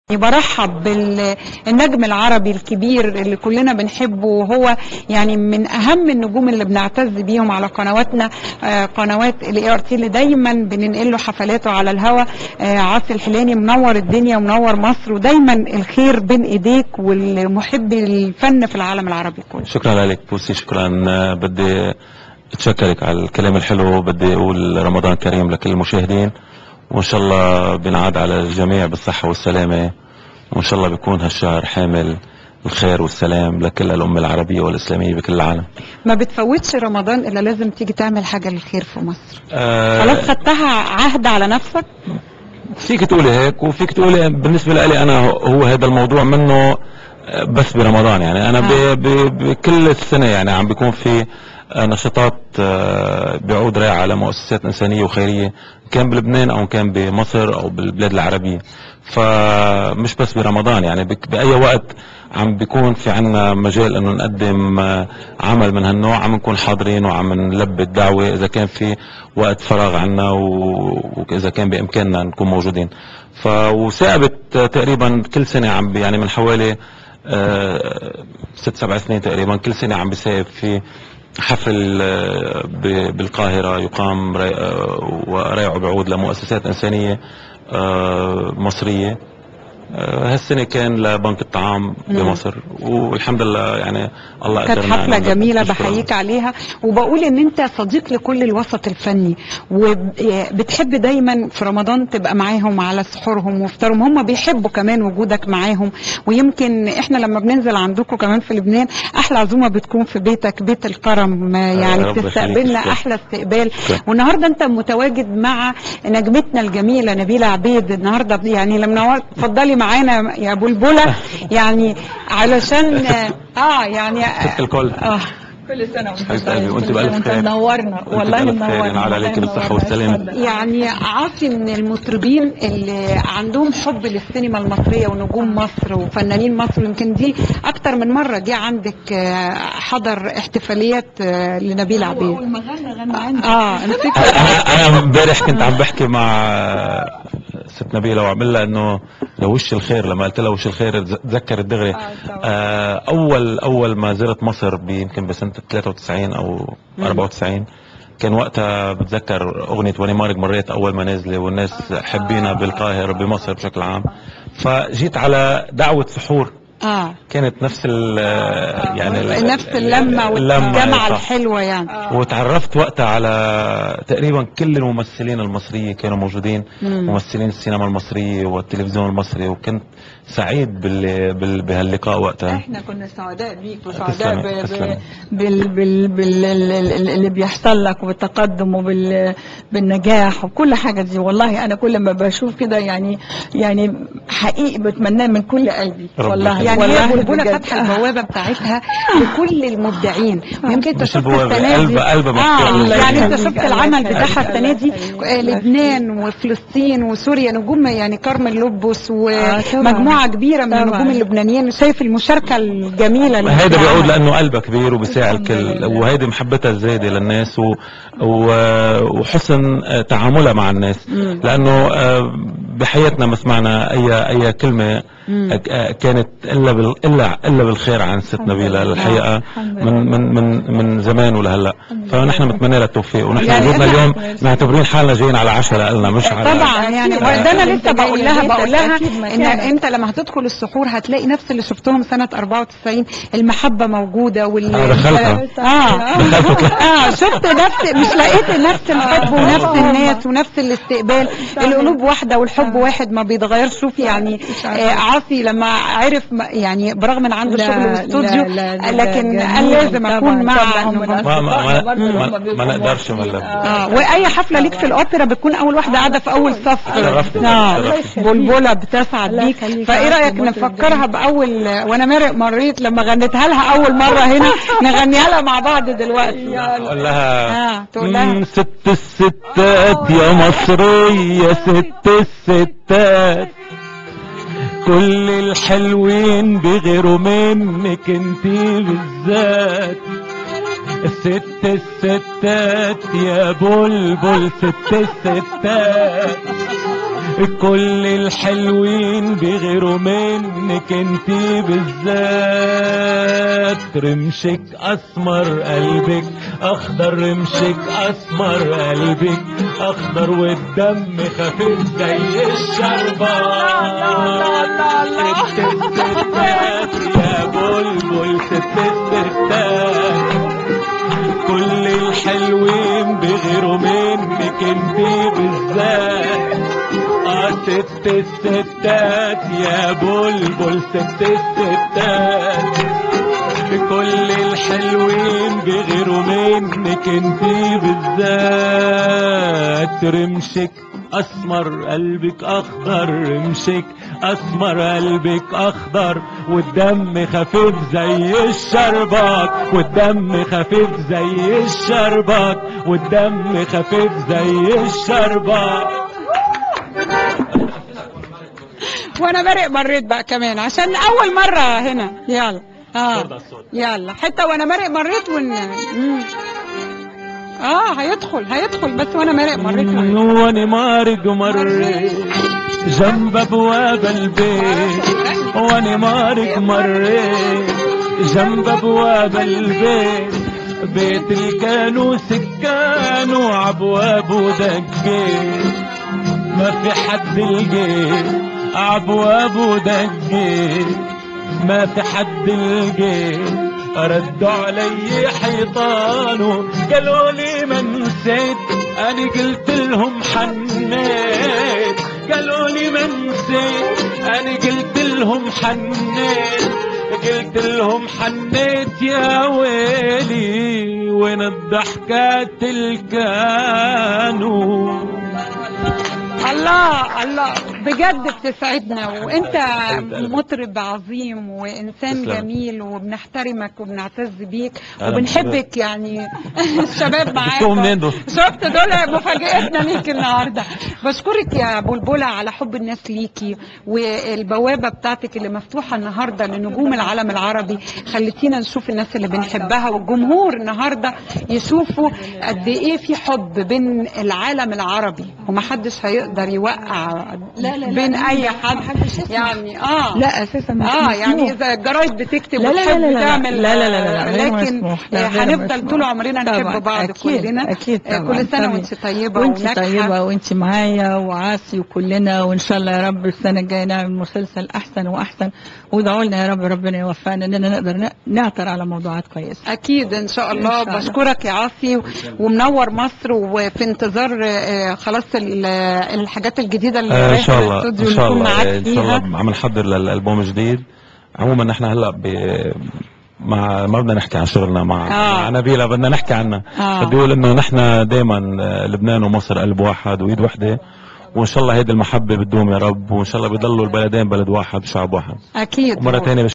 Assi El Hallani & Nabila Obeid | لقاء عاصي الحلاني و نبيلة عبيد
Assi El Hallani & Nabila Obeid | 2009 | لقاء عاصي الحلاني و نبيلة عبيد في دعوة علي السحور